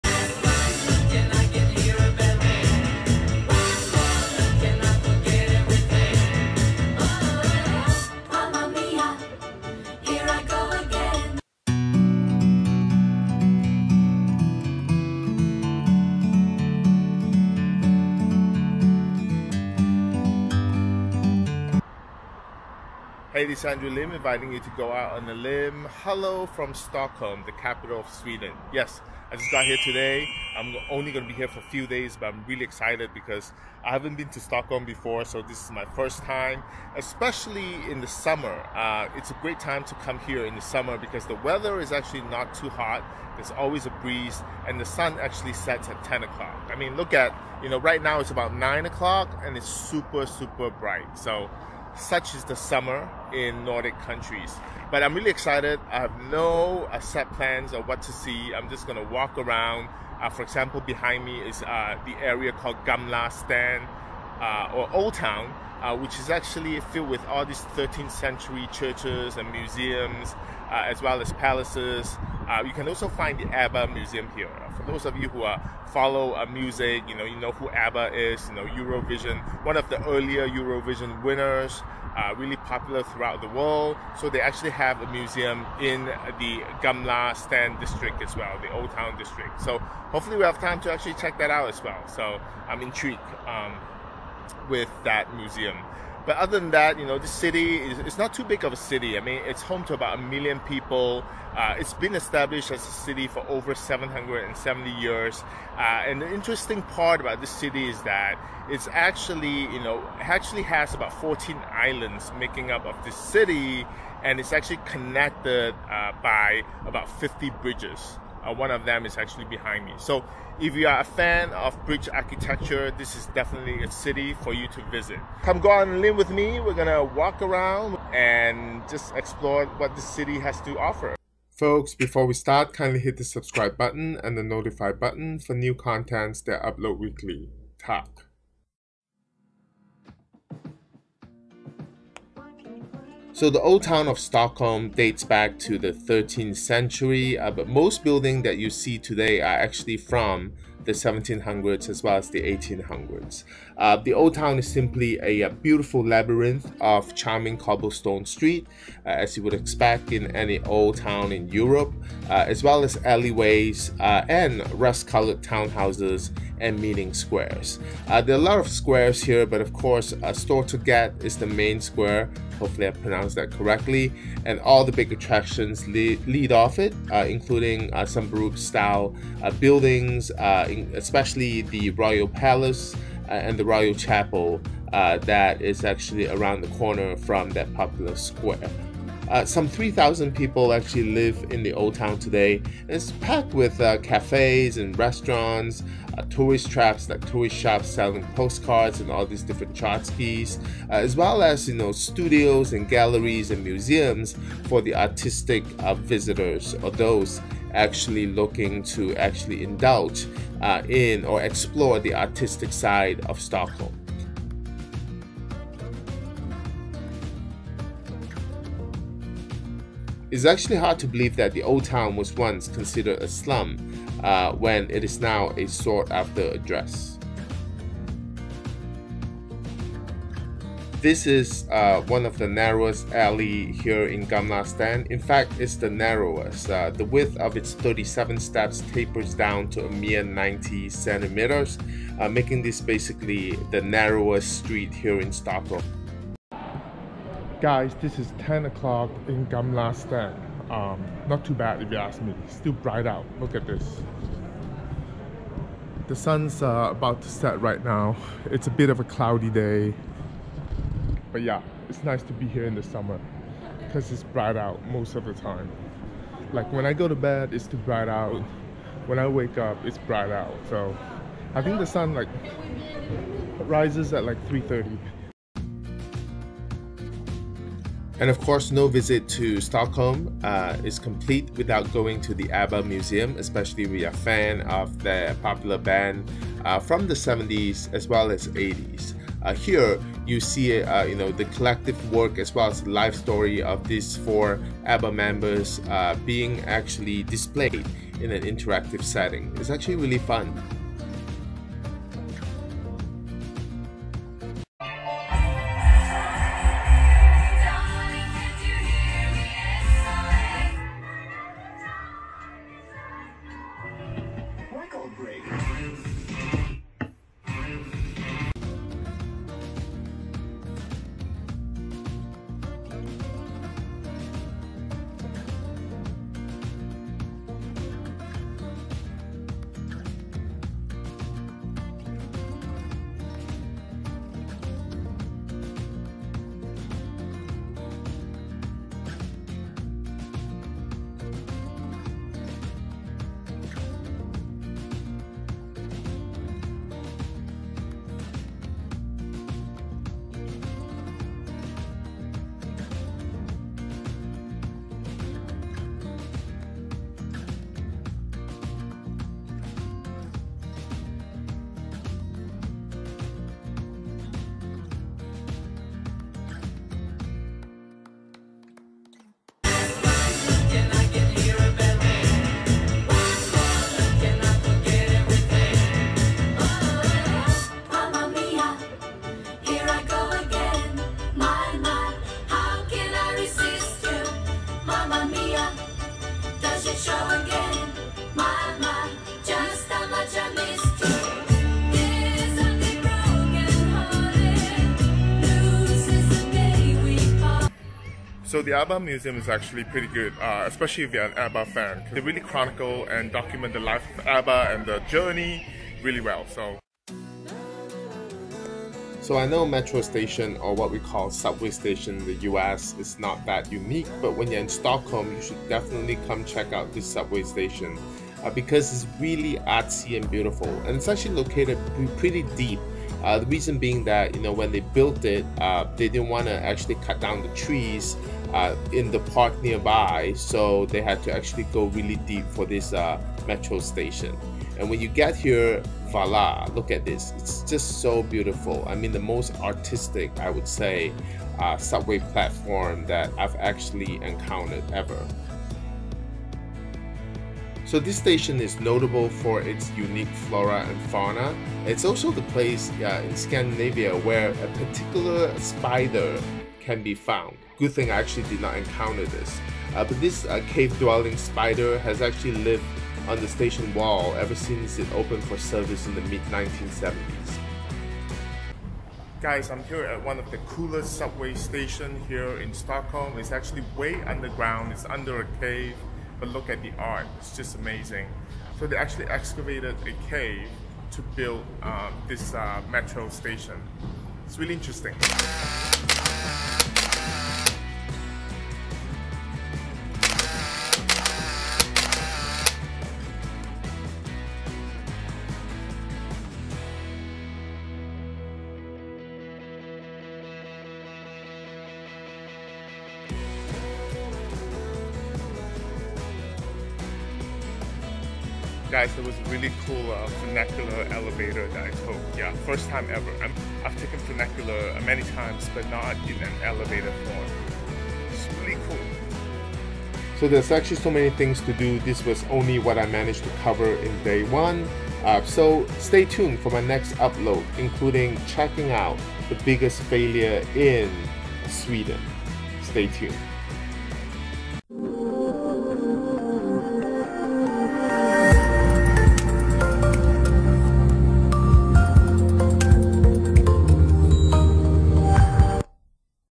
Abba Museum, Stockholm Old Town and Most Colorful Underground Metro in Stockholm
As the summer is nearing its end, come enjoy a typical Scandinavian summer day with me by walking on the cobbled streets of Gamla Stan, Stockholm's oldest part of town adorned with colorful 17th and 18th century buildings, admire the most artsy Subway platform underground carved out of a cave, and appreciate the pride and joy of Sweden that is Abba and learn about the famous pop band's road to fame.